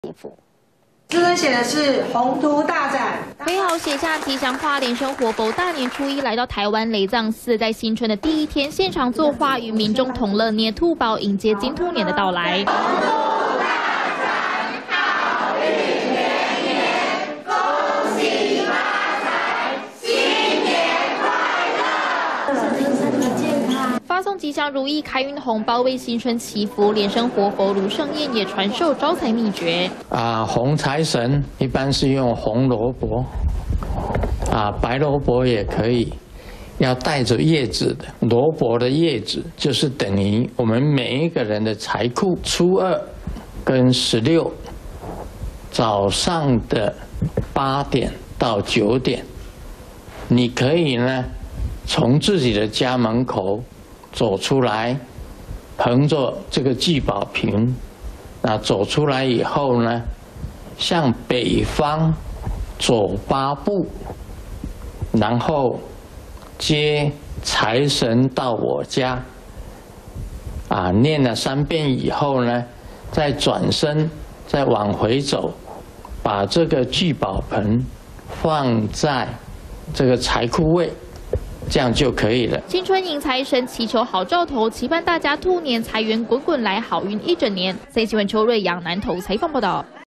鴻「兔」大展迎新春！蓮生活佛揮毫開運祈福 親授招財秘訣│三立iNEWS
迎接金兔年到來！蓮生活佛盧勝彥大年初一，在南投雷藏寺發送「吉祥如意開運紅包」，並現場作畫、揮毫，與民眾「捏兔寶」同樂，為新春祈福！